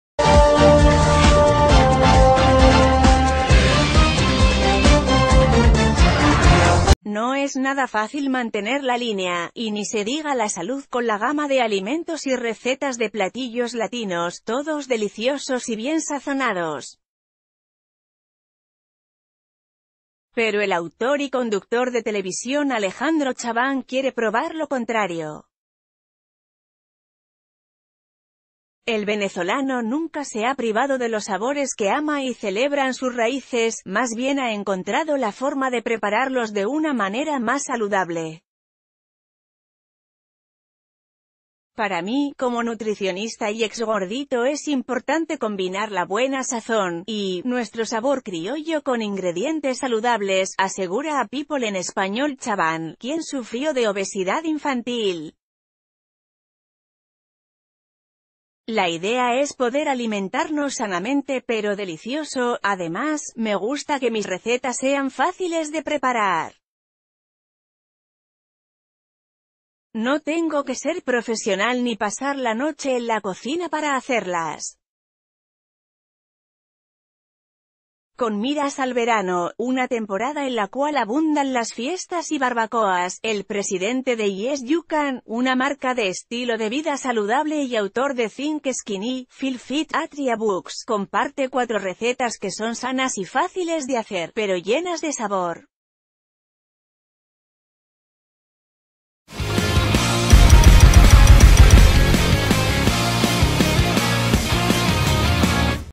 Alejandro Chabán comparte 4 recetas sanas y sabrosas
No es nada fácil mantener la línea —y ni se diga la salud —con la gama de alimentos y recetas de platillos latinos, todos deliciosos y bien sazonados. Pero el autor y conductor de televisión Alejandro Chabán quiere probar lo contrario. El venezolano nunca se ha privado de los sabores que ama y celeb...